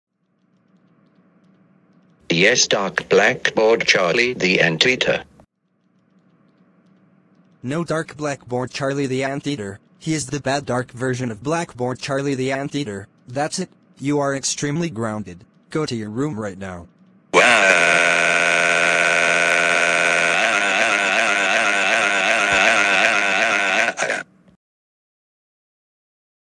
Dark Blackboard Charlie The Anteater Jr Says Yes Dark Blackboard Charlie The Anteater/Grounded
Dark Blackboard Charlie The Anteater Jr Says Yes Dark Blackboard Charlie The Anteater And Got Grounded By Black King Gummy Bear.

Don't Remove The Video, Don't Copyright The Video, And Don’t Put Age Restriced On This